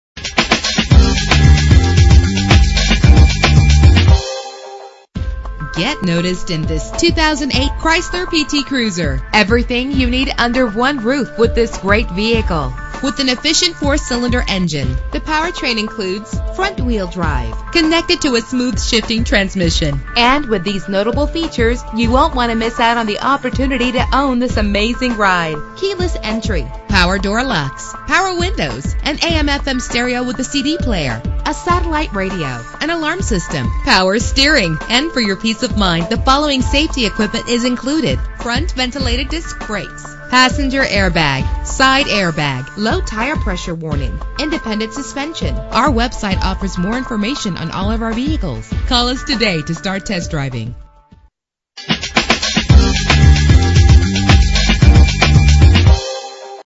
2008 Chrysler PT Cruiser - Tampa FL
Year: 2008
Make: Chrysler
Model: PT Cruiser
Trim: LX
Engine: 2.4 liter inline 4 cylinder 16 valve
Transmission:
Color: White
Mileage: 46168
Address:
7201 West Linebaugh Avenue